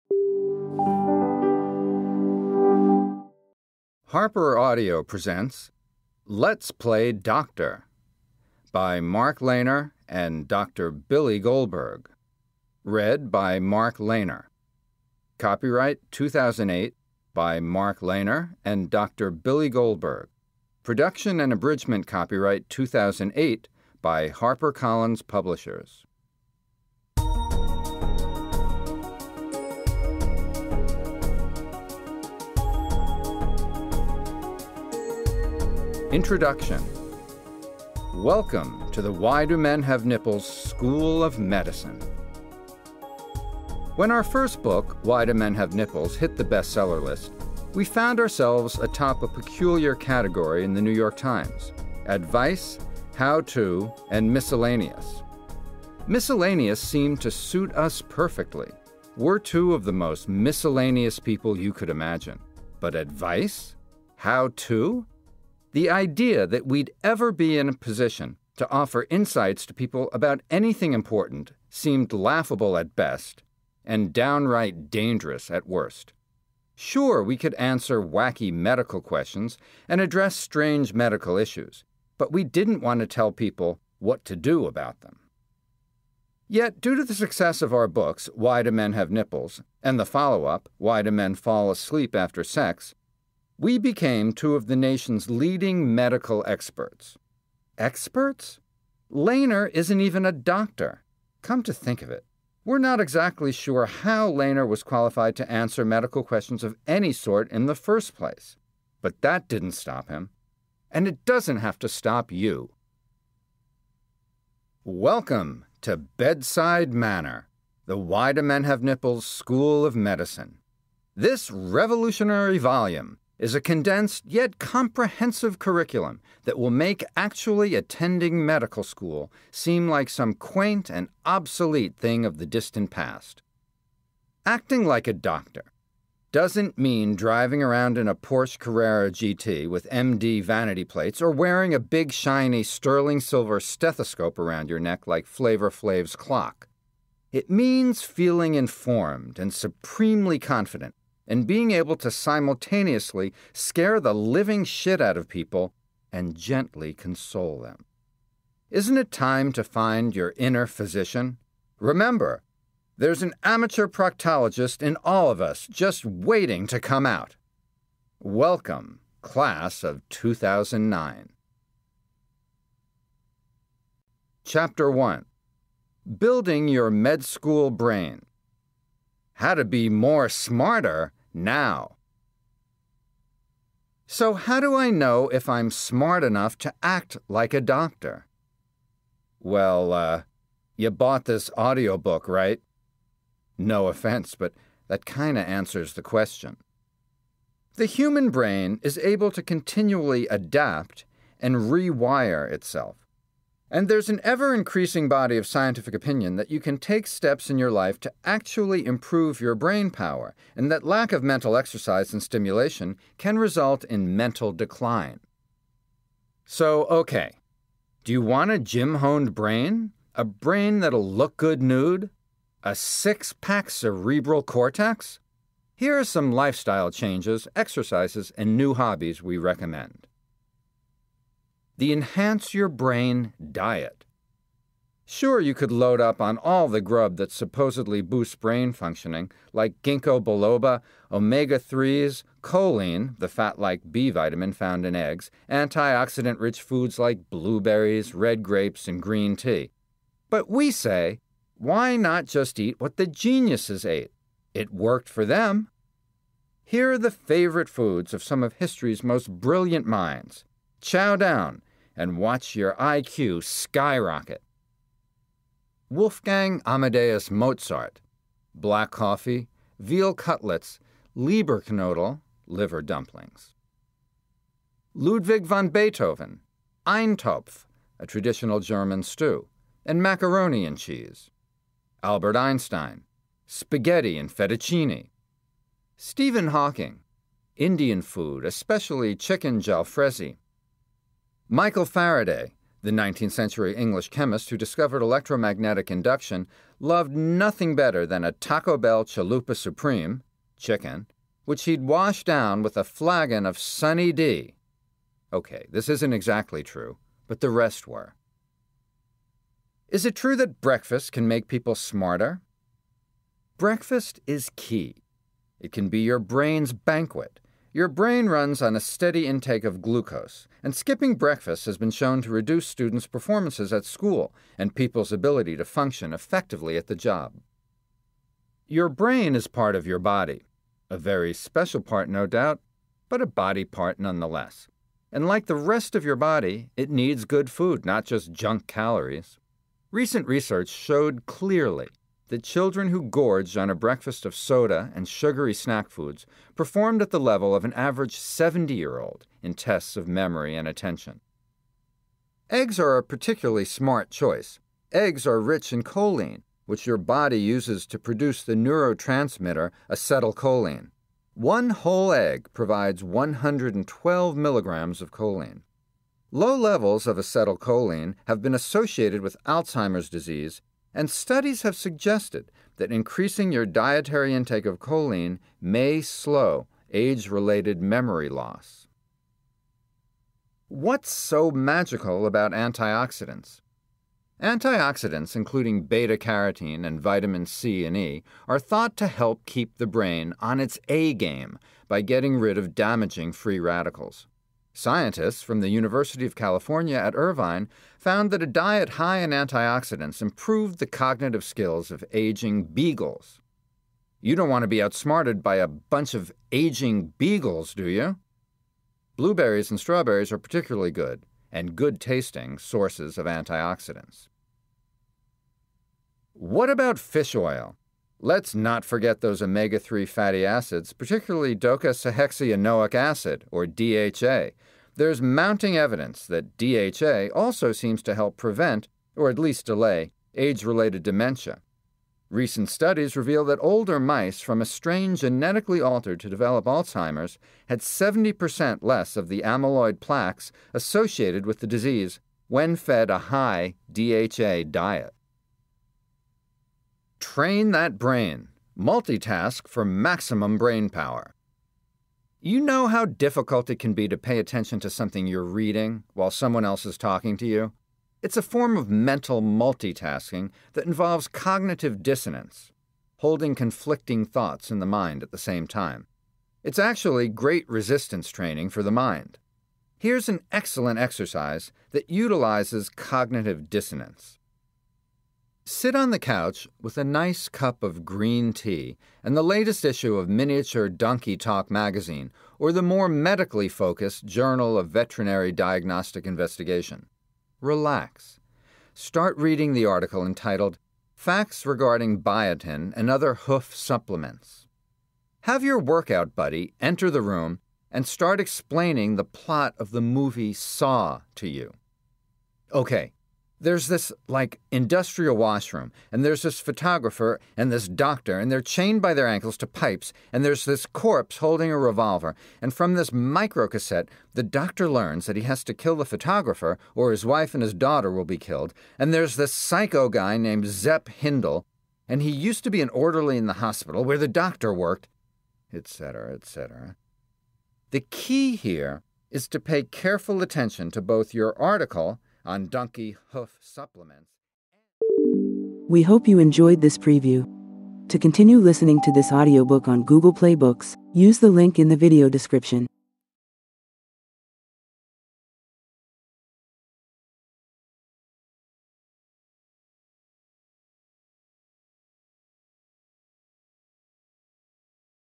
Let's Play Doctor by Mark Leyner · Audiobook preview
Let's Play Doctor
Authored by Mark Leyner, Billy Goldberg
Narrated by Mark Leyner
Abridged

BOOK DESCRIPTION

Let's Play Doctor is your instant guide to becoming a Real Fake Doctor. At the Why Do Men Have Nipples School of Medicine, we offer an informative, immersive, and incredibly entertaining course of study that will give you the special skills needed to get your M.D. on! By following the lessons in Let's Play Doctor, you'll learn:  Special mental exercises to give yourself that buff, bulging Doctor brain  How to impress your peers with big, polysyllabic, esoteric medical lingo (can you say pneumonoultramicroscopicsilicovolcanokoniosis?)  Easy ways to diagnose your girlfriend's goiter or your father's fistula  Do-it-yourself surgeries from hemorrhoidectomy to breast enlargement  And, most important, how to craft a completely believable, official-sounding get-out-of-work-for-medical-reasons note

Mark Leyner is the author of My Cousin, My Gastroenterologist; Tooth Imprints on a Corn Dog; I Smell Esther Williams; Et Tu Babe; and The Tetherballs of Bougainville. He has written scripts for a variety of films and television shows. His writing appears regularly in The New Yorker, Time, and GQ.

Billy Goldberg, M.D., is an emergency medicine physician on faculty at a New York City teaching hospital. He is also a writer and artist whose paintings have been exhibited in New York City.

AUDIOBOOK DETAILS

Language: English
Publisher: HarperAudio
Published on: December 2, 2008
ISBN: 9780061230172
Duration: 1 hr, 25 min
Genres: Humor / General